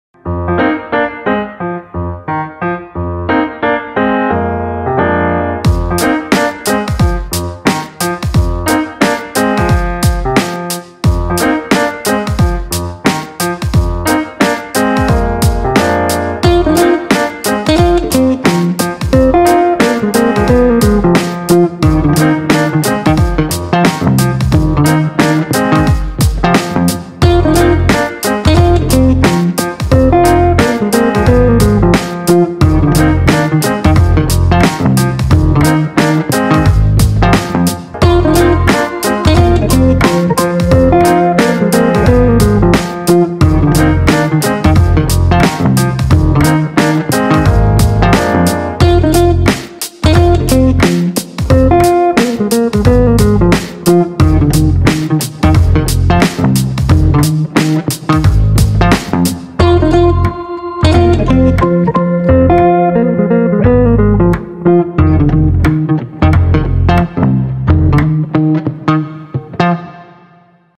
Настольная лампа EGLO 49718 - видеообзор
Divine-light.ru представляет видеообзор товаров настольная лампа EGLO серии Barnstaple.